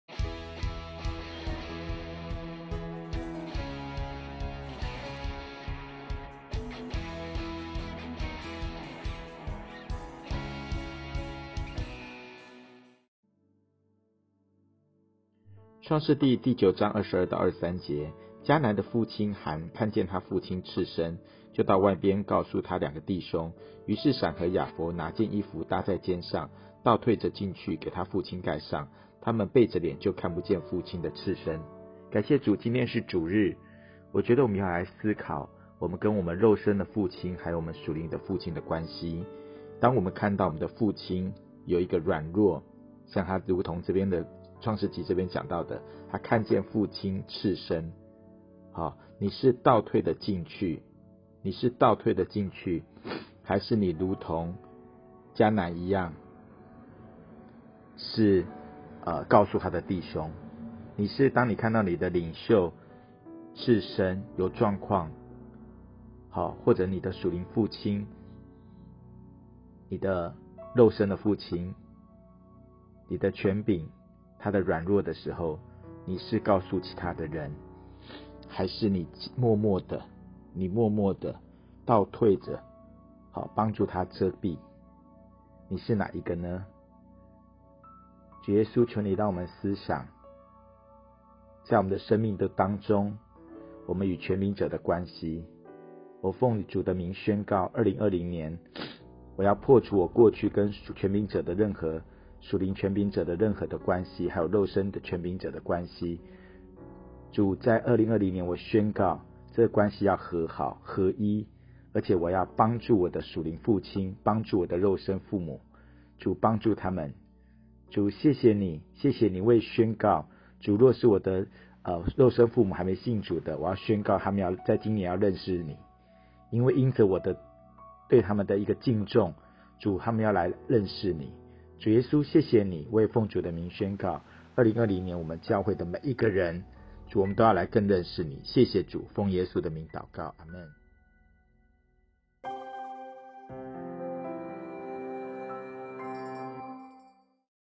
「每日靈糧：創世記8-9章」迦南的父親含，看見他父親赤身，就到外邊告訴他兩個弟兄。於是閃和雅弗，拿件衣服搭在肩上，倒退著進去，給他父親蓋上，他們背著臉就看不見父親的赤身。（創9:22-23 ）
「22.迦南的父親含，看見他父親赤身，就到外邊告訴他兩個弟兄。23. 於是閃和雅弗，拿件衣服搭在肩上，倒退著進去，給他父親蓋上，他們背著臉就看不見父親的赤身。」（創9:22-23 ）